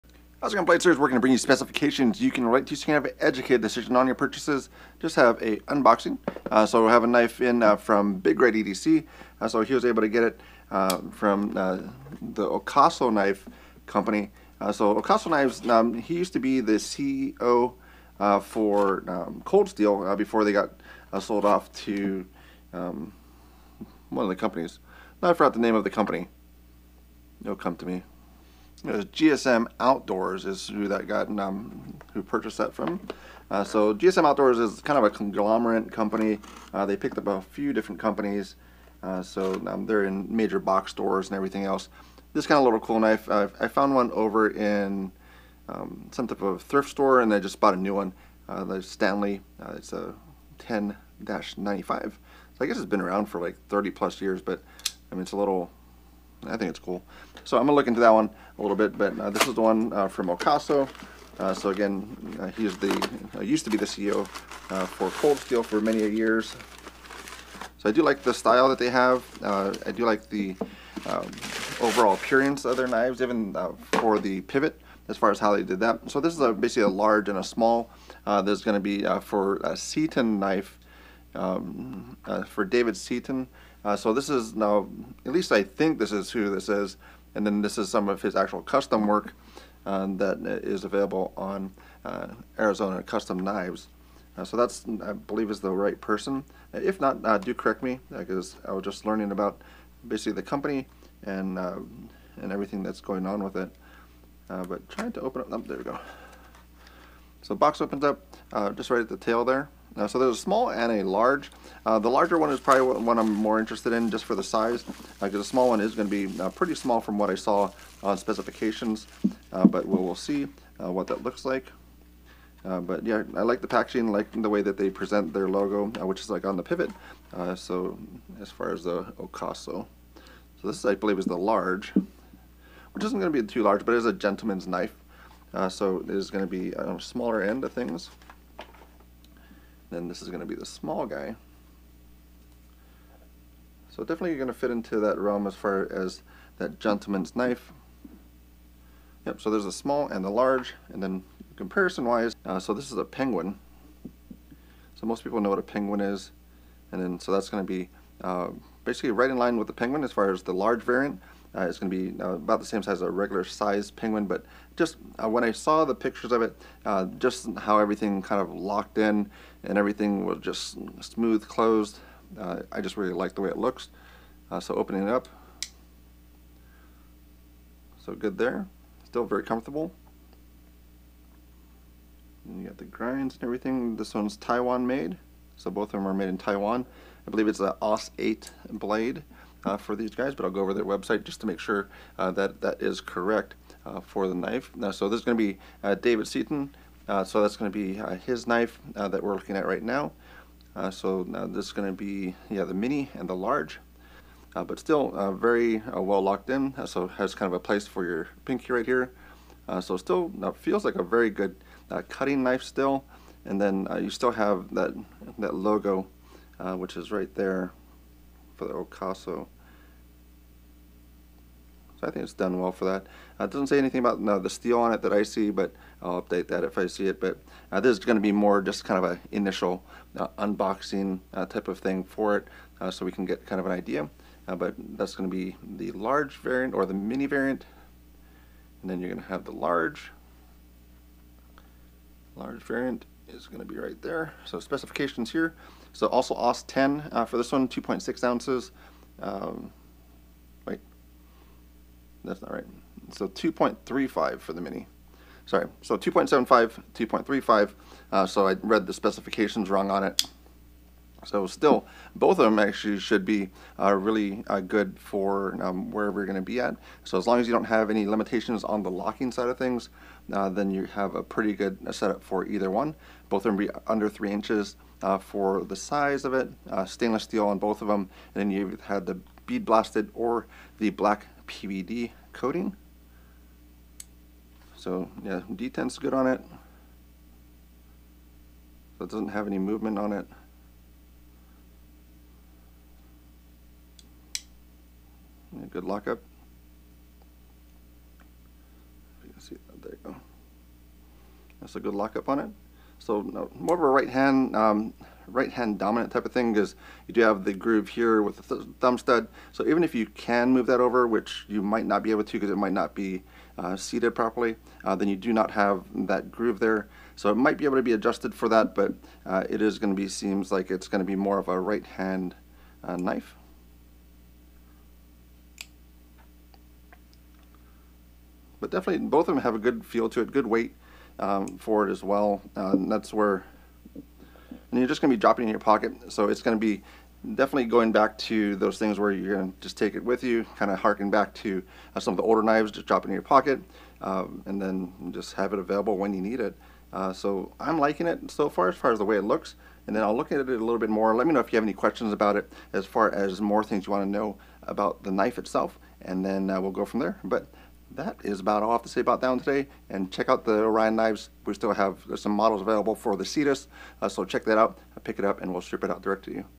Ocaso Seaton Gentleman's Knives Mini and Large
Thank you to Big Red EDC for sending these along from Ocaso Knives. I really liked the look of the knives when i saw photos of them.